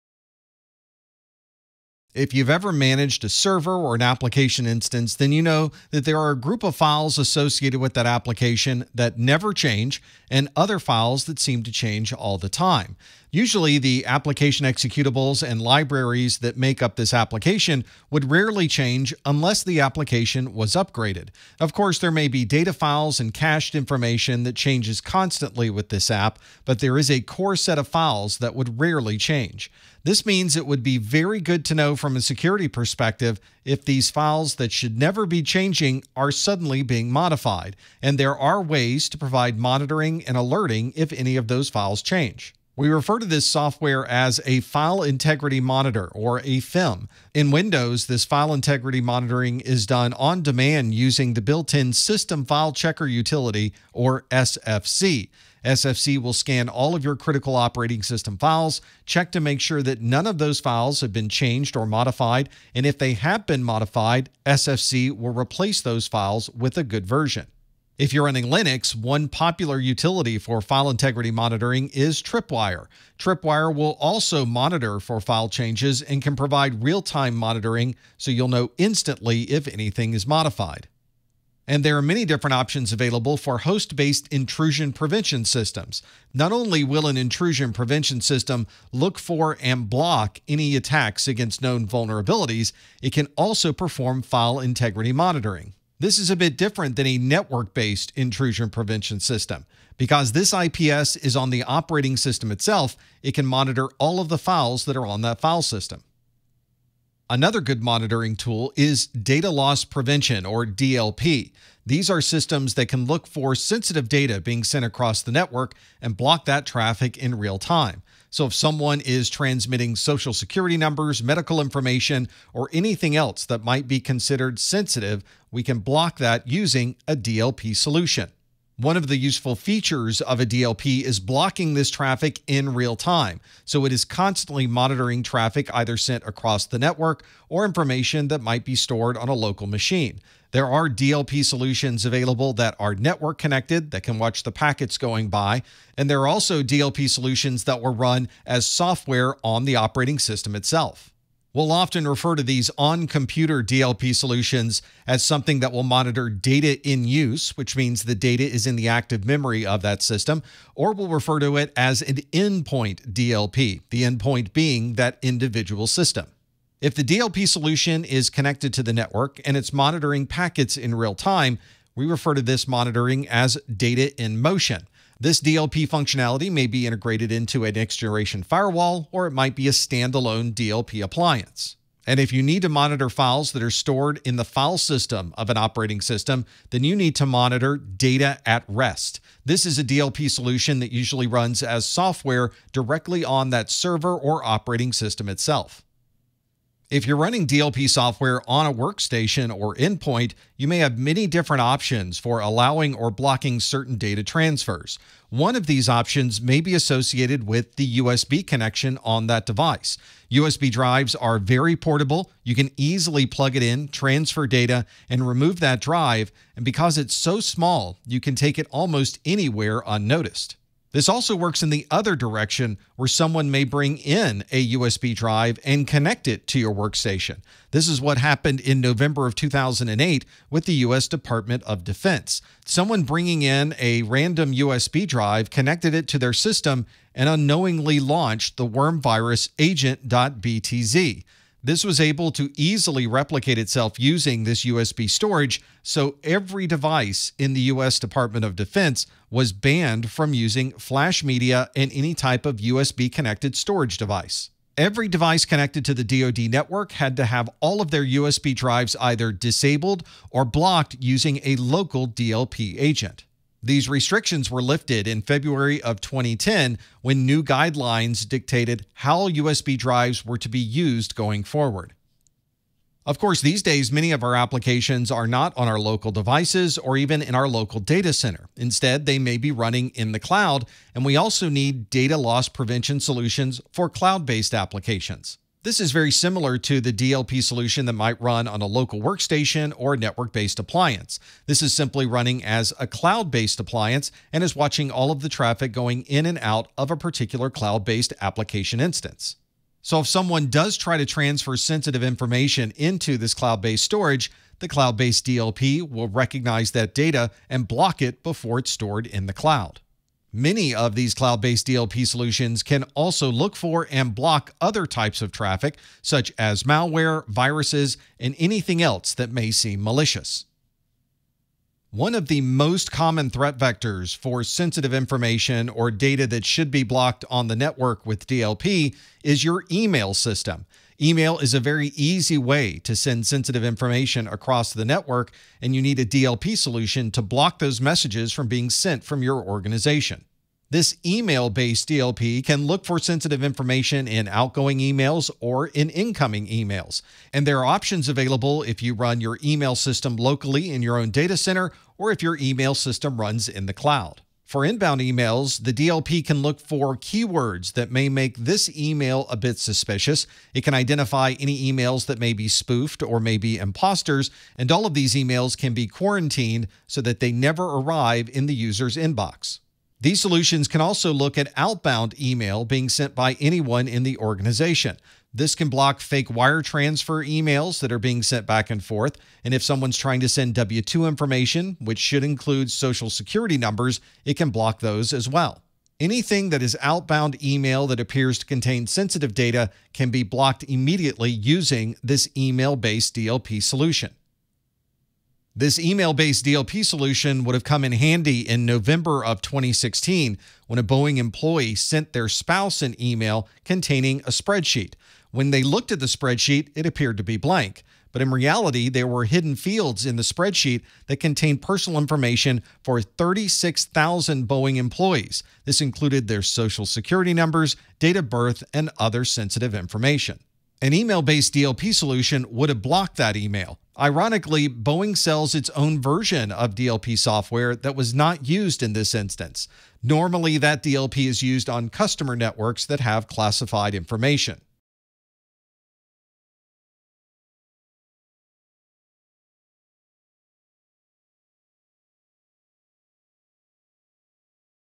Monitoring Data - CompTIA Security+ SY0-701 - 4.5
A good offense against an attacker is to have a good defense. In this video, you'll learn about data loss prevention, file integrity monitoring, USB blocking, and more.